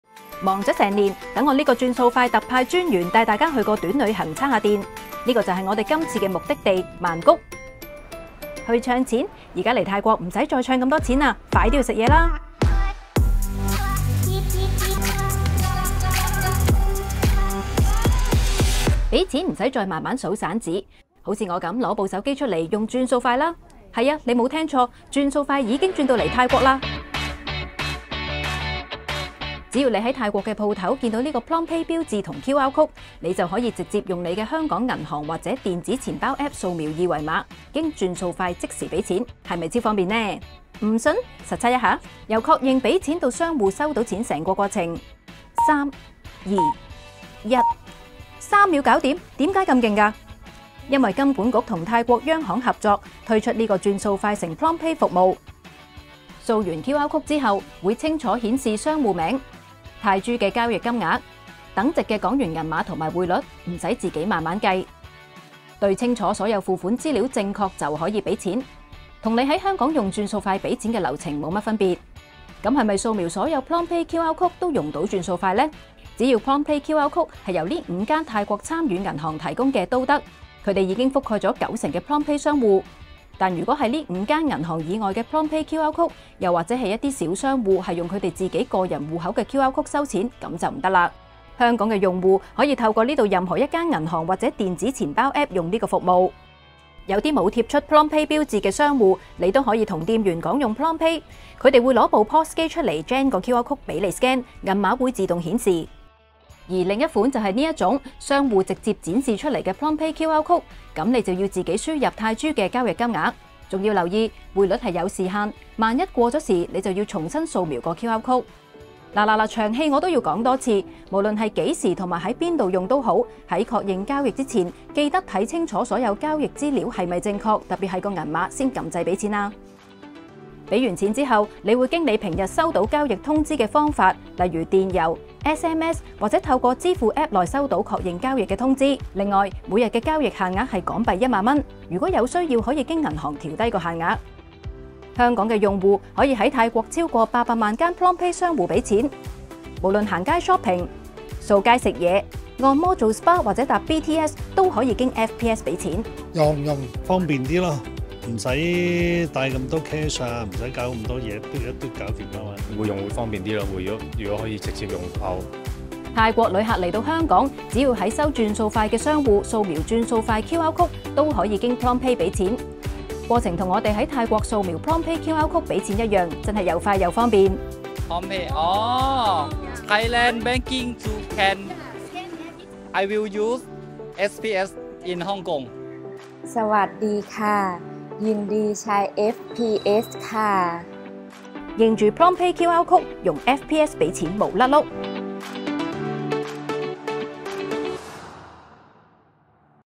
轉數快 x PromptPay: 轉數畀錢泰EASY ｜FPS x PromptPay: Pay Easy with FPS in Thailand
轉數快特派專員帶大家去實地測試一下「轉數快 x PromptPay」這個新服務，一齊睇下有幾快同幾方便啦!

The FPS ambassador will take everyone to test the new service "FPS x PromptPay” on the ground.  Let's see how fast and convenient it is!